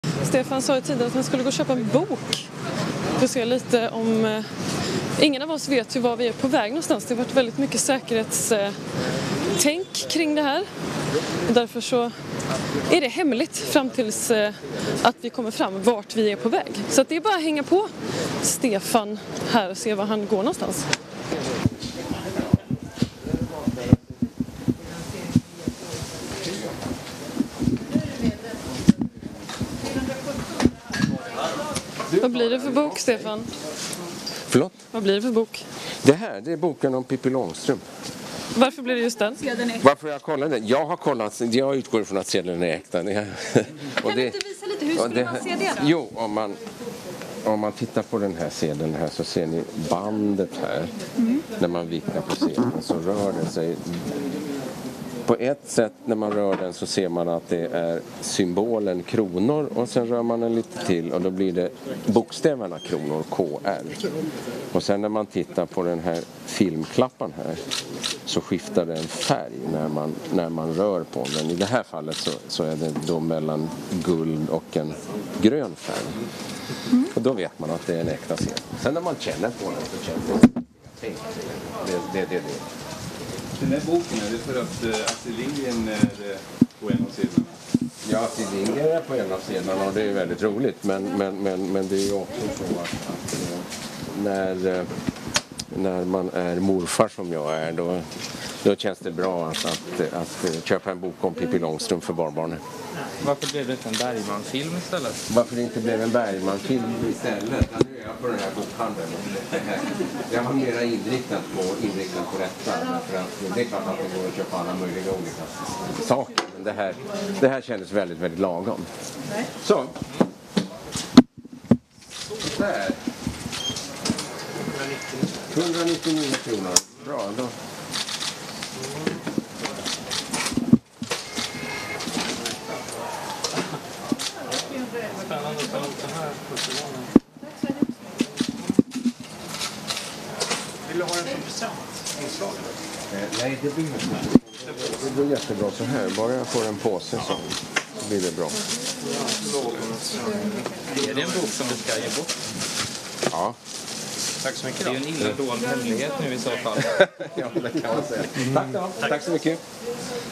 Riksbankschefen Stefan Ingves handlar Pippi Långstrump-bok med nya sedeln
Stefan Ingves besökte Akademibokhandeln i Stockholm